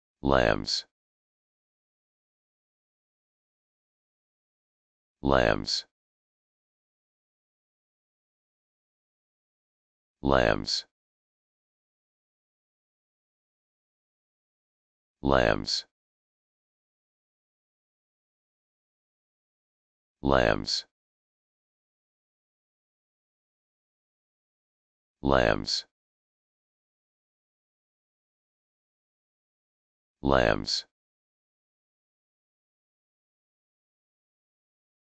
How to Pronounce Lambs
Lambs - your guide to pronouncing this word, brought to you for free by uPronounce.